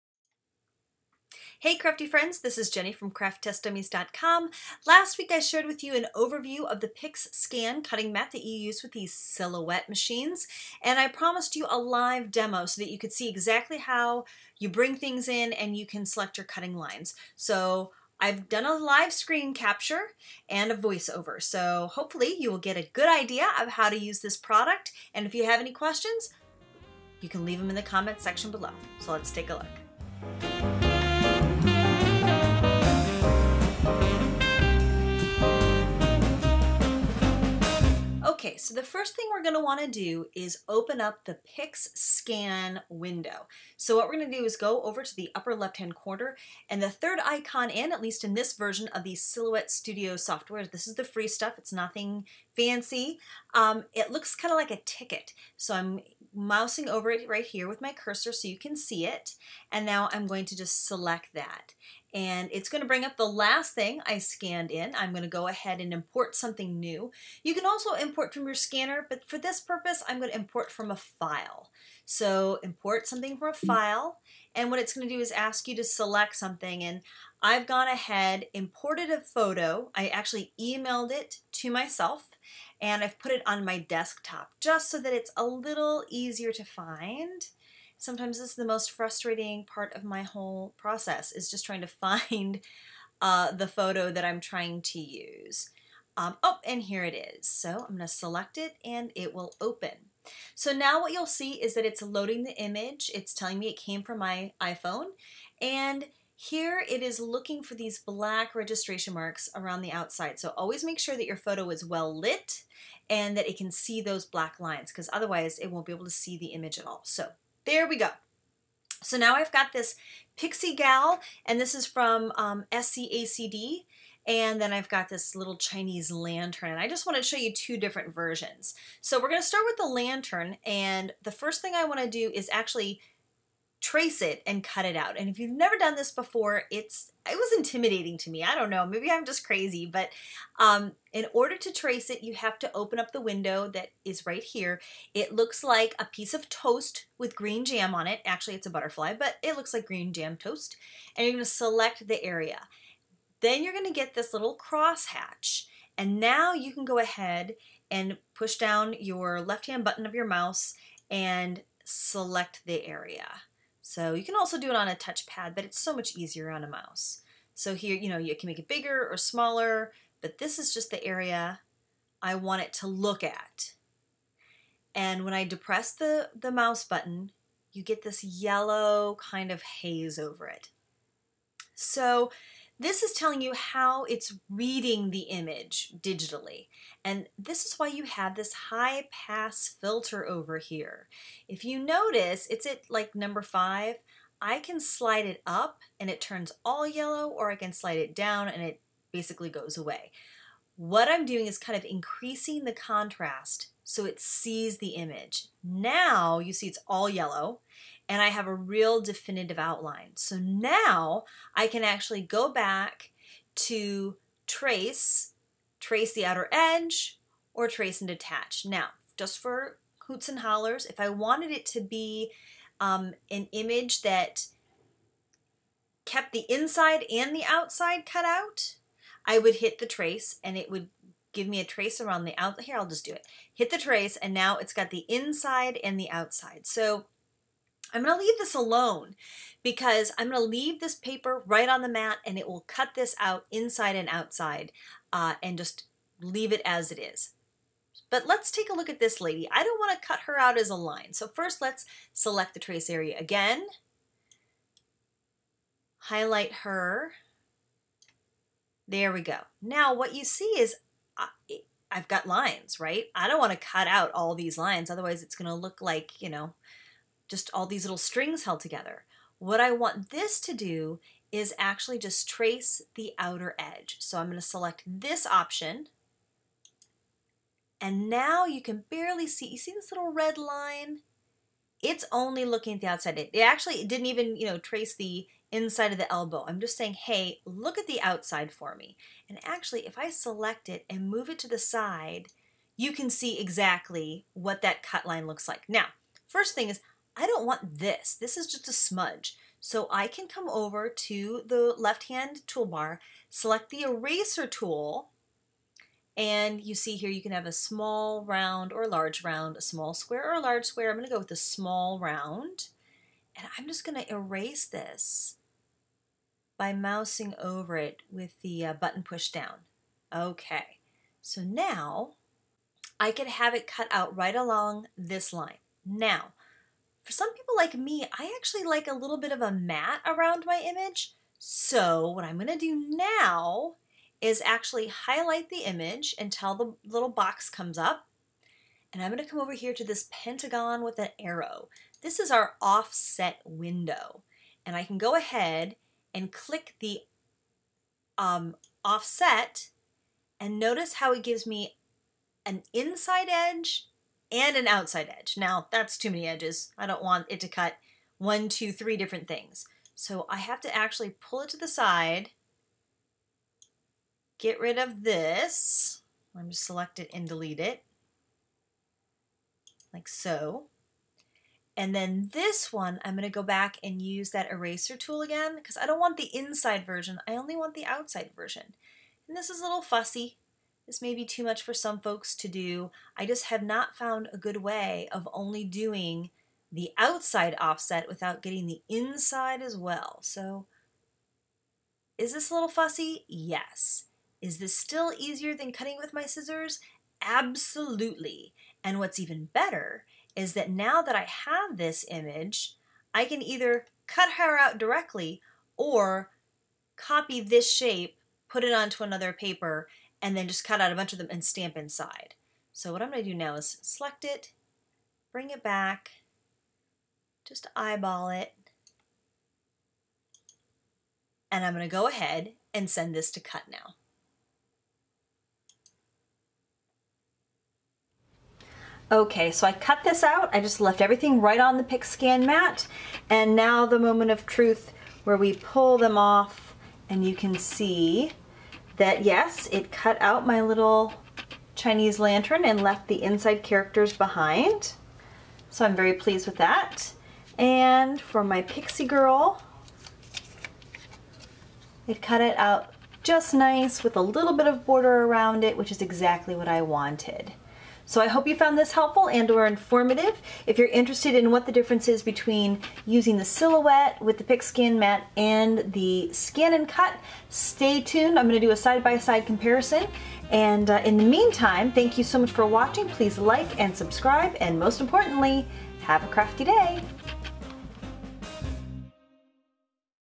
Using the PixScan with your Silhouette Studio Software
Today we are using the PixScan with your Silhouette Studio Software. In this LIVE screen capture video, I walk you through how to import your file, which icons to use, how to trace, and even adding an offset (border) around a stamped image.